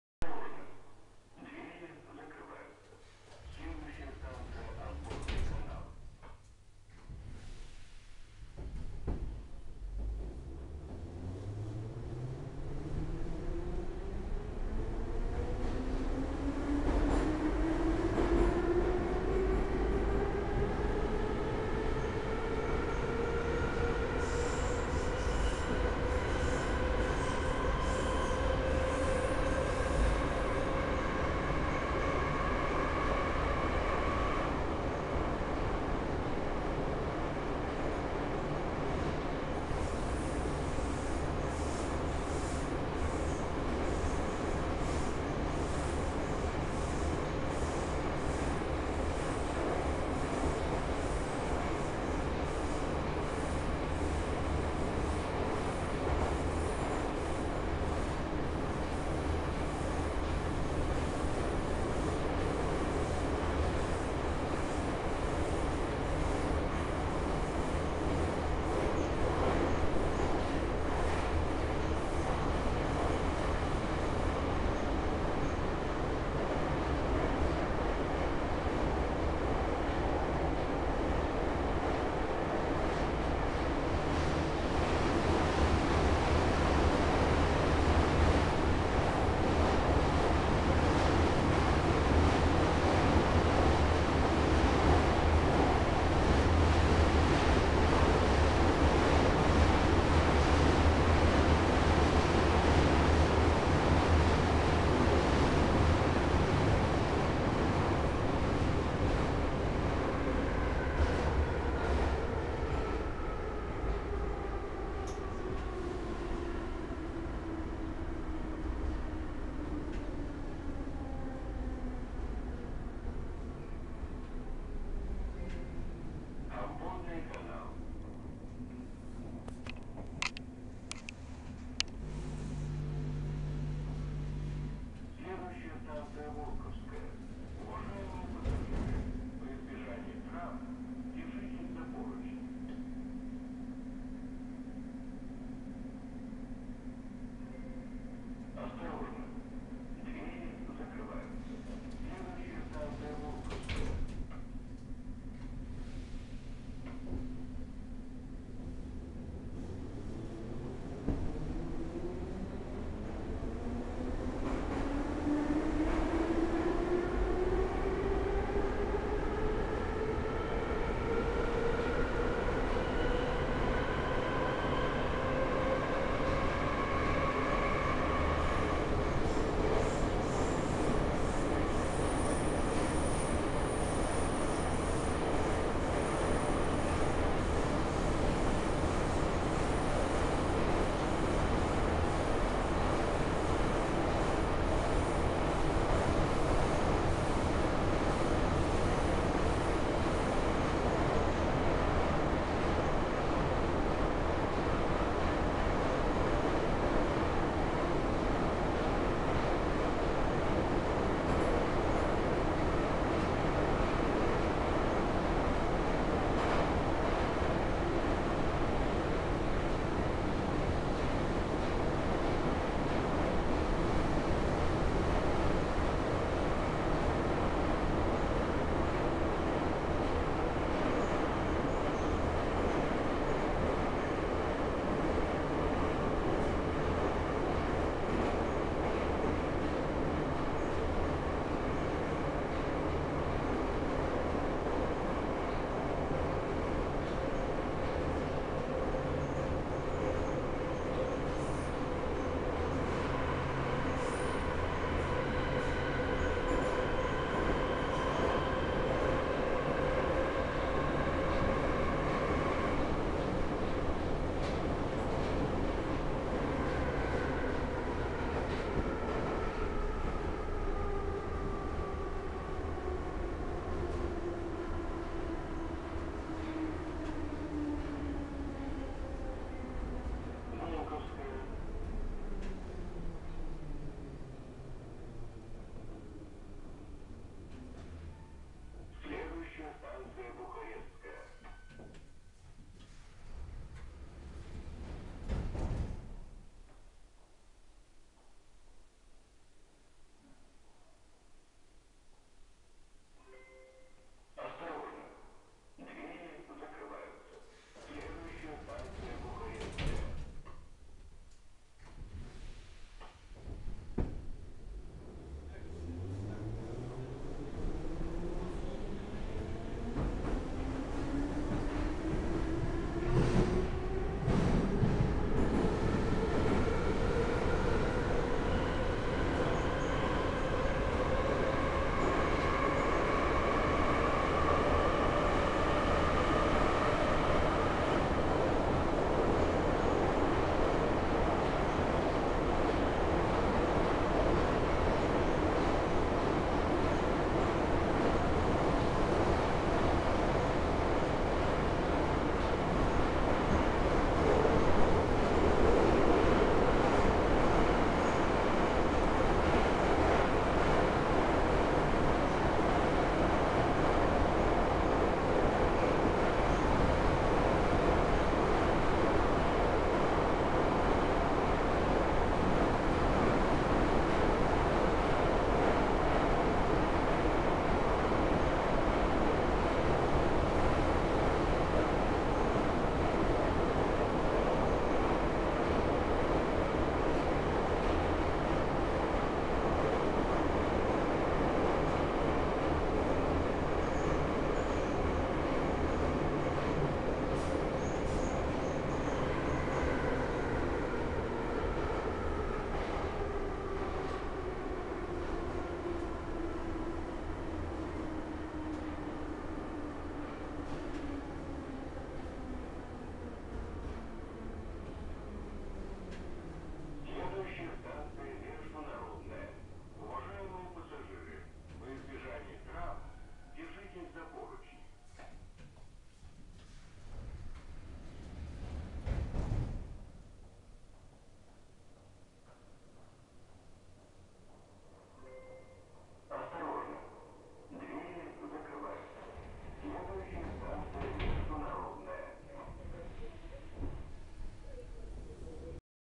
Saint-Petersburg Metro M5 (FPL) - 81-717.5 10148
Zvënigorodskaja - Buharëstskaja
1:52 - Obvodnyj Kanał
4:29 - Vołkovskaja
6:42 - Buharëstskaja